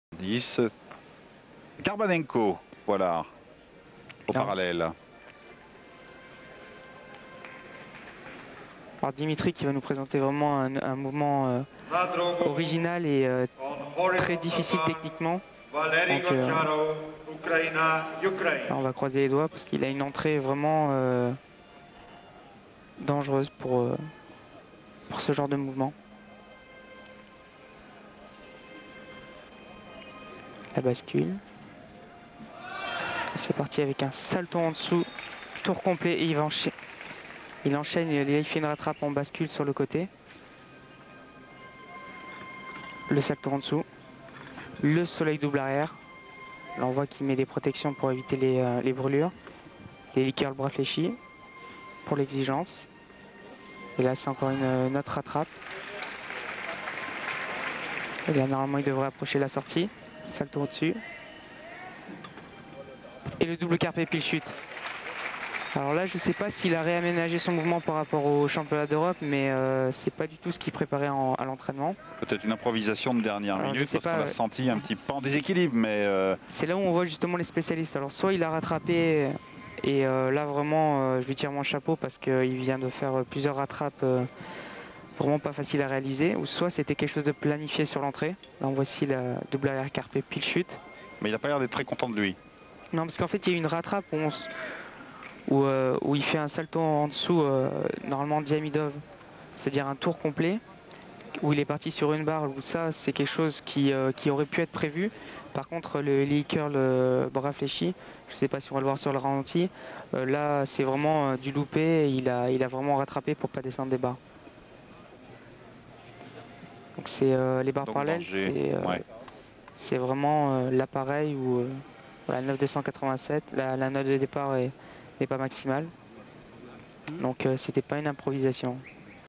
Dimitri KARBANENKO (FRA) PB - 2004 Europeans Ljubljana Team
Dimitri KARBANENKO (FRA)

'04 European championships in Ljubljana - Team competition

9.287 (SV: 9.8)
Team rank : 3rd